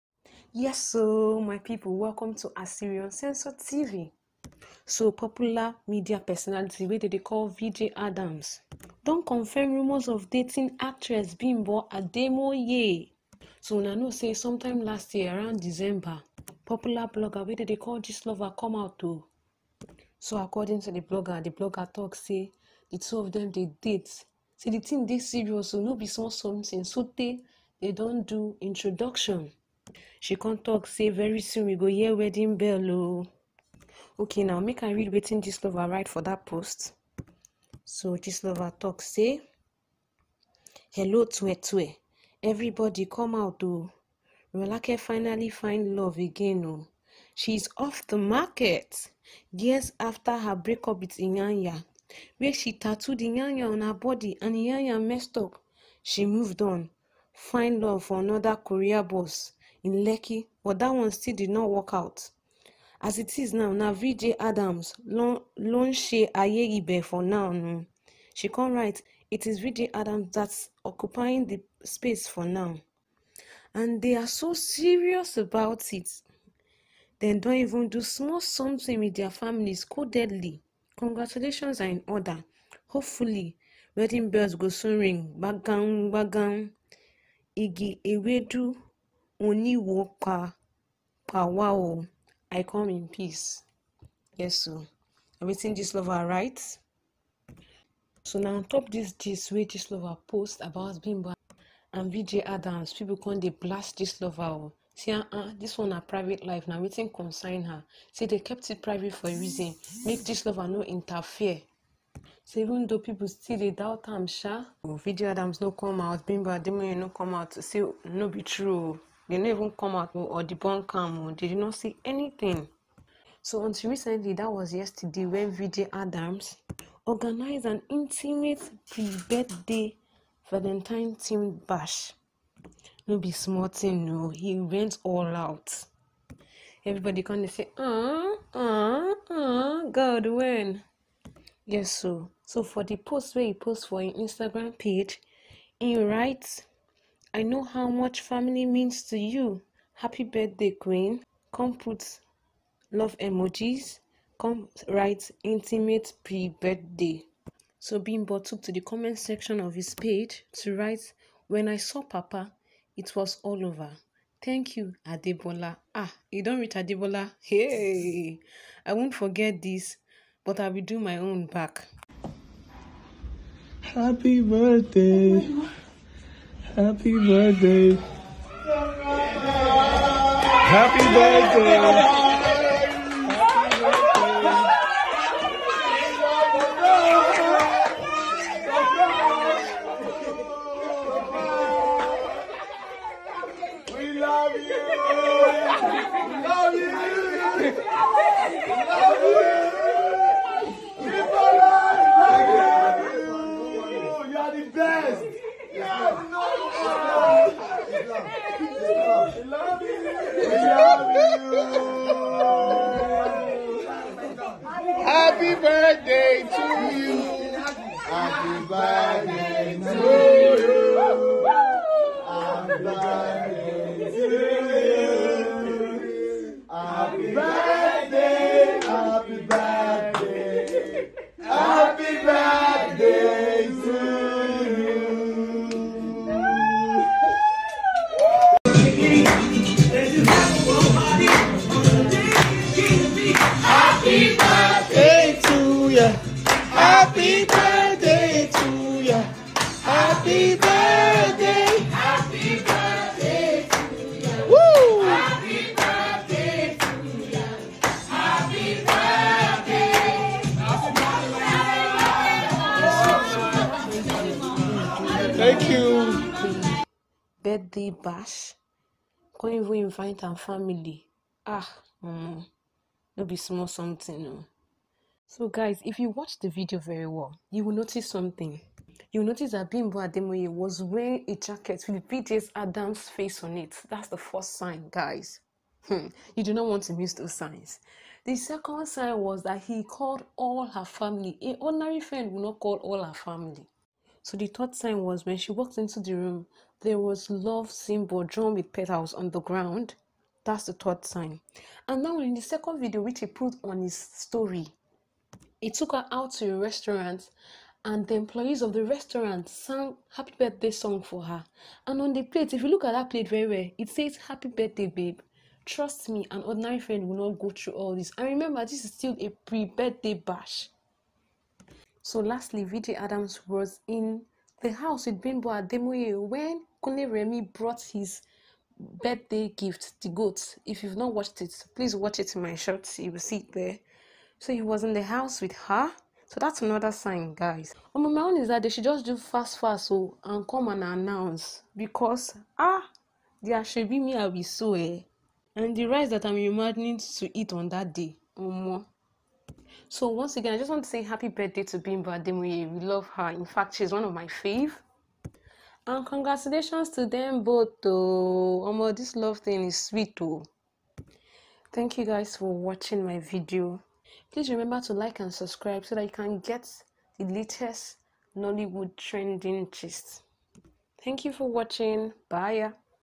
😲VJ Adams & Actress Bimbo Ademoye Confirms Engagement
Bimbo Ademoye & VJ Adams Confirms Rumors Of Dating